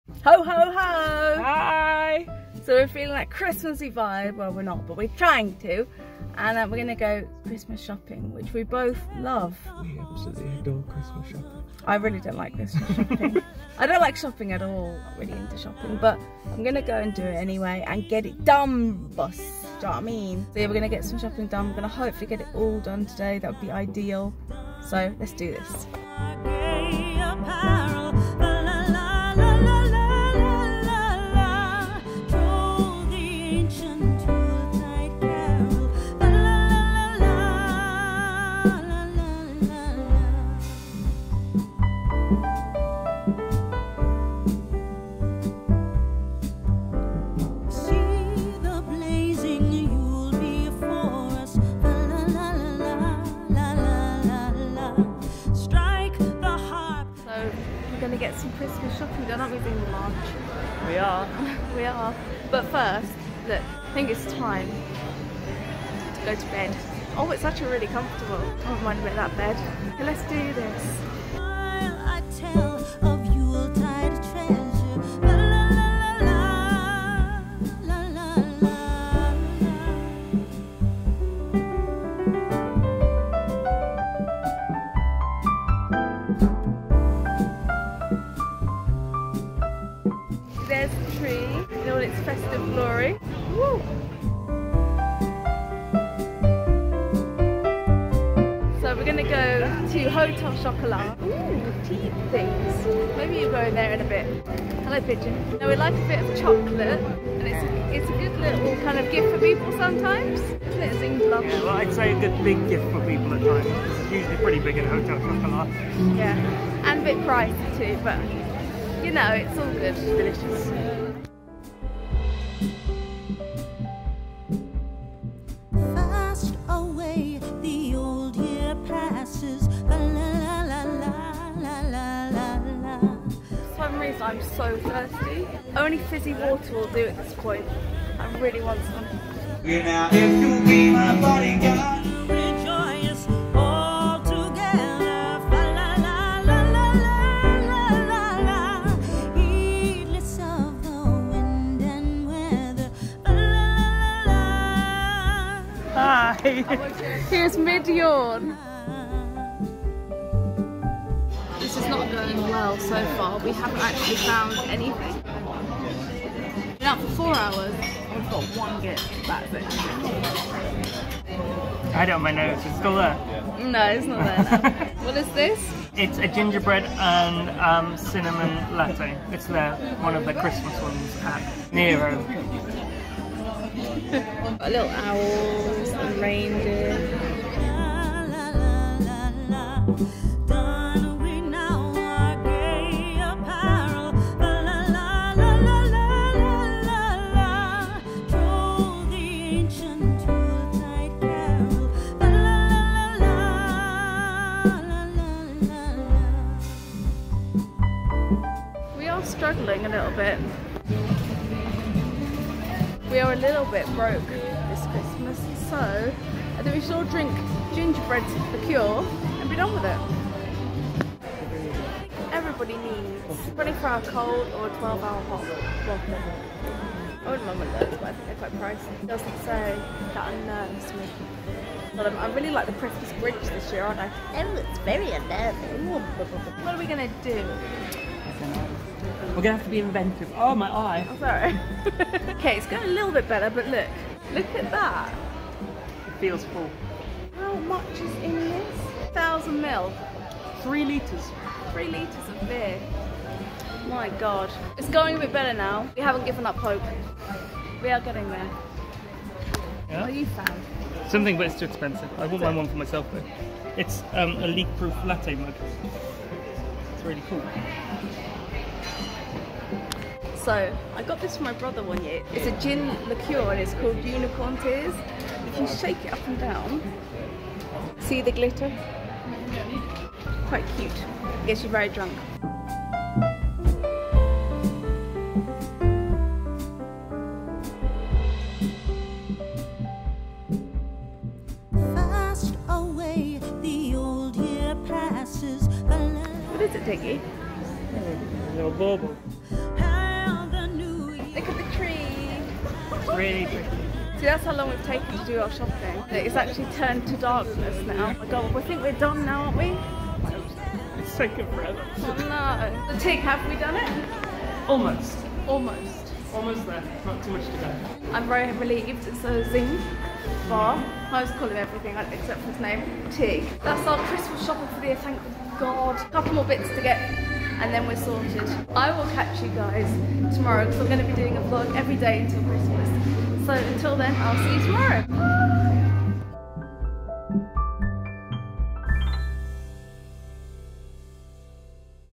LETS CHRISTMAS SHOP
So today we hit the streets of Kingston in London to get our Christmas shopping done... tig and me at the best of times arent a fan of shopping at all but we managed to nail most of it in one shop woo!